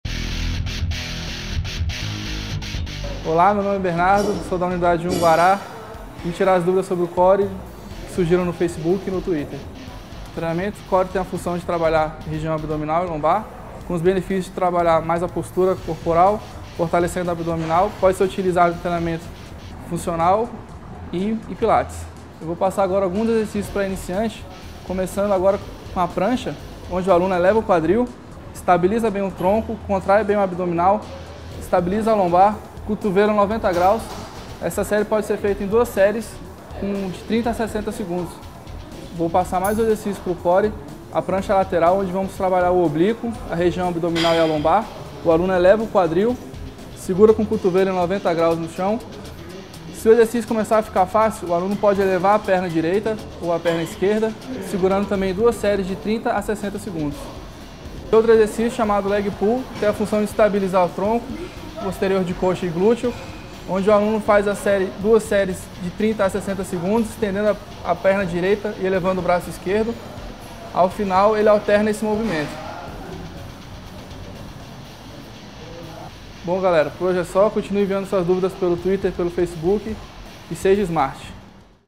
Dicas Core - Smart Fit
Dicas para exercício de Core com o professor Bernardo da unidade Guará.

Aprimore seu treino de core com as melhores dicas da Smart Fit. Descubra como realizar um treino de core completo para fortalecer o centro do seu corpo e melhorar sua estabilidade.

00:00 Introdução
00:10 Para que serve o core
00:22 Exercícios iniciantes
01:16 Seja Smart